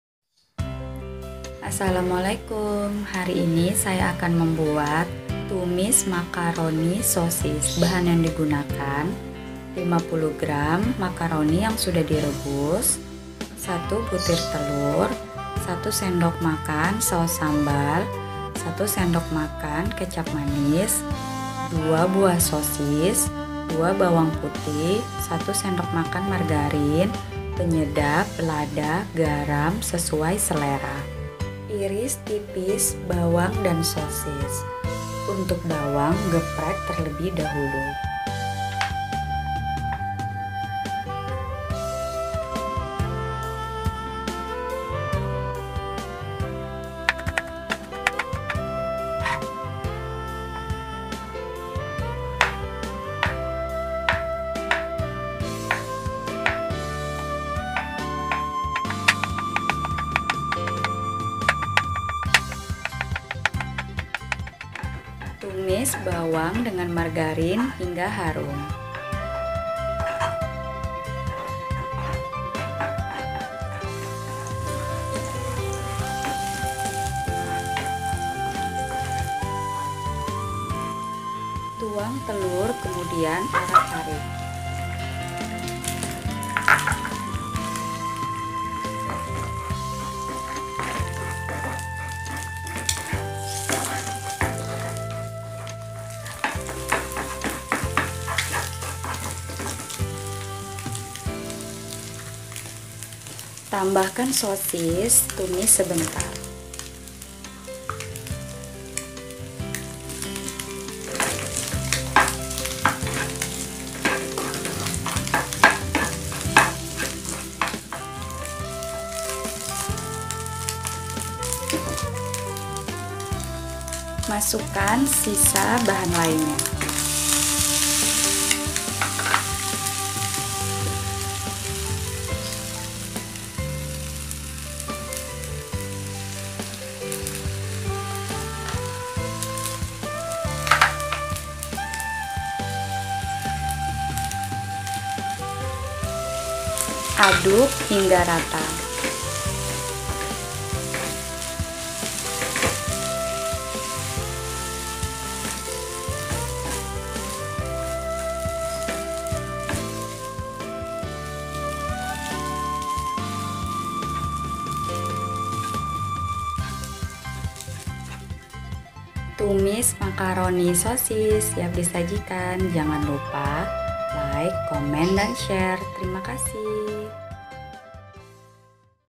Tumis Makaroni Sosis | Belajar Membuat
Bahan - bahan yang digunakan :

- 50 gr makaroni yg sdh direbus
- 2 sosis
- 1 butir telur
- 1 sdm margarin
- 2 bawang putih
- 1 sdm kecap manis,saos sambel
- 1/2 sdt garam, lada,penyedap